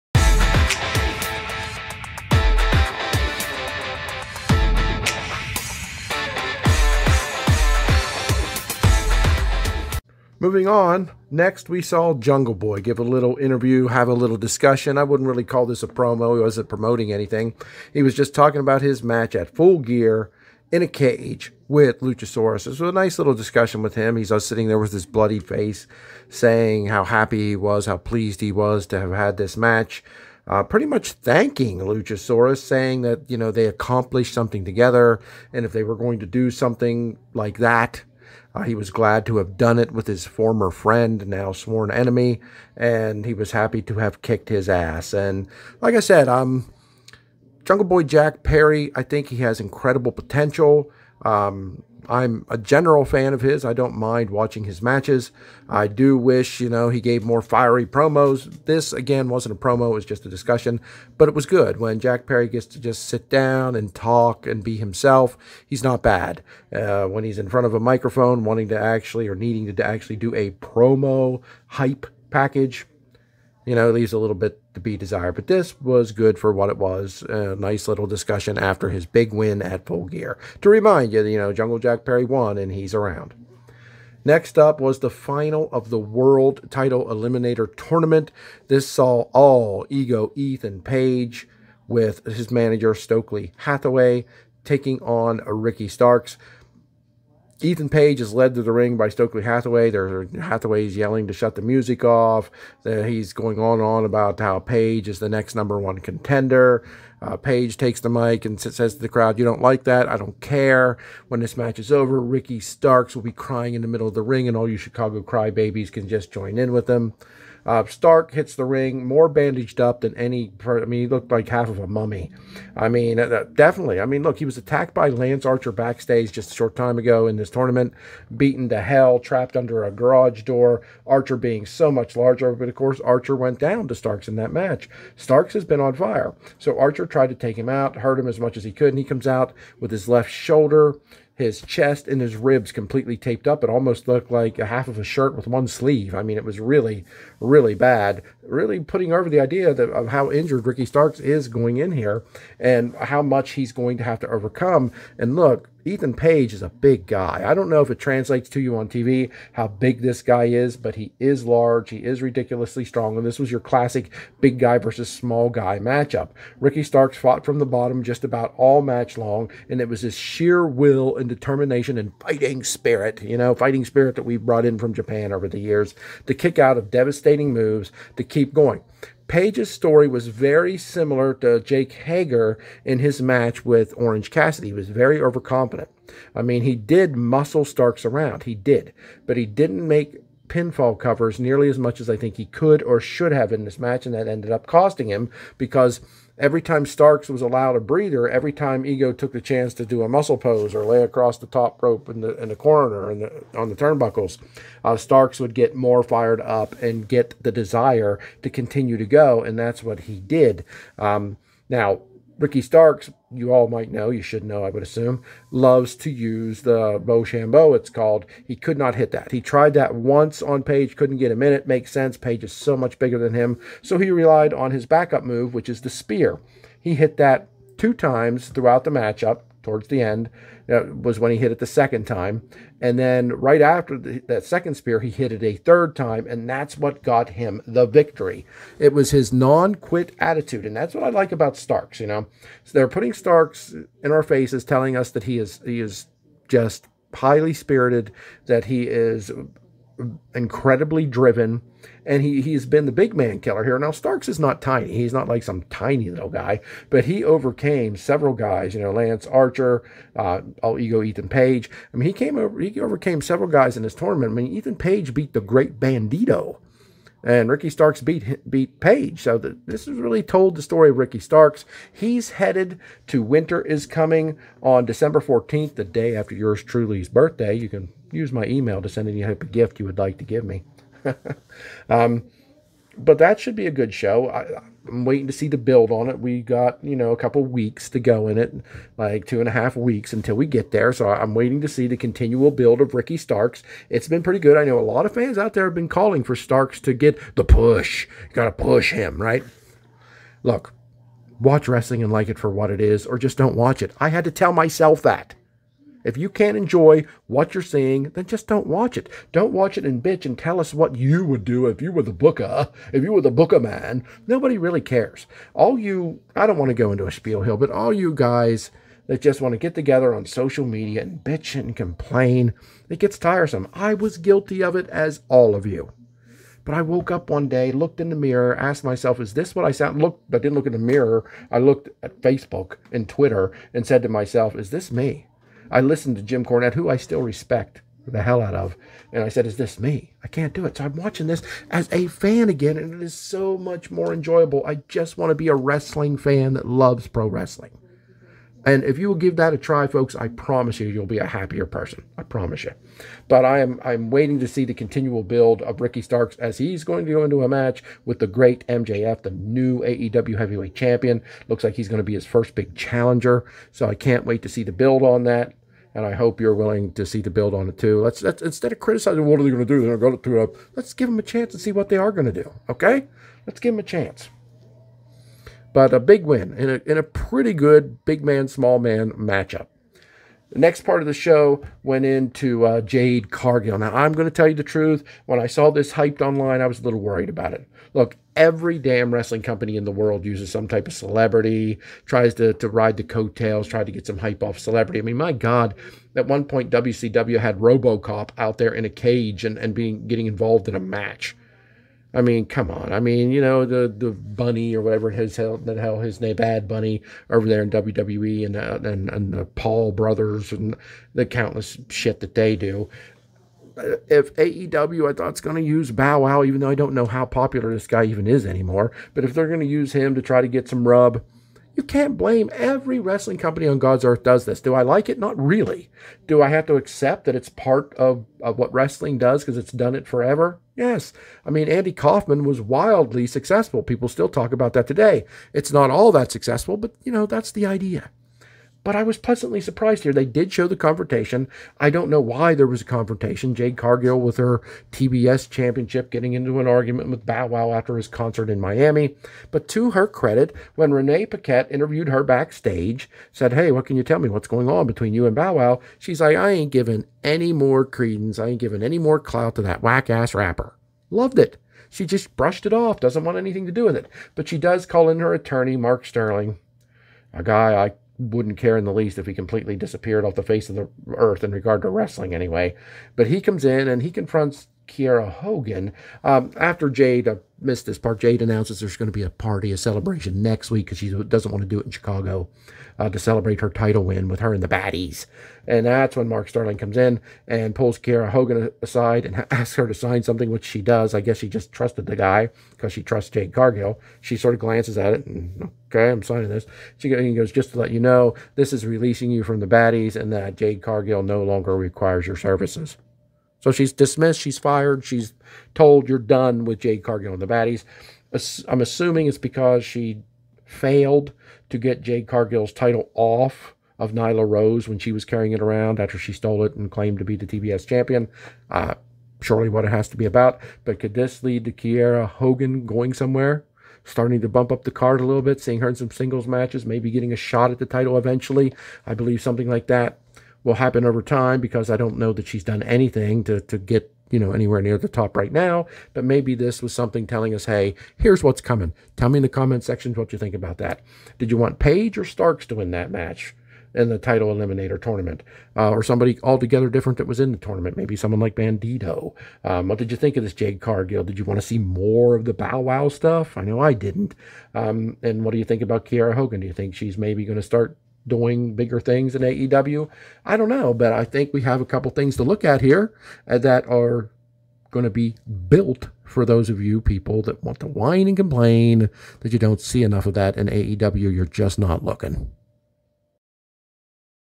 Jungle Boy Interview, World Title Eliminator Final, Jade Cargil and Bow Wow Thoughts
Quick thoughts from Jungle Boy
Ethan Page vs Ricky Starks
Jade Cargill on Bow Wow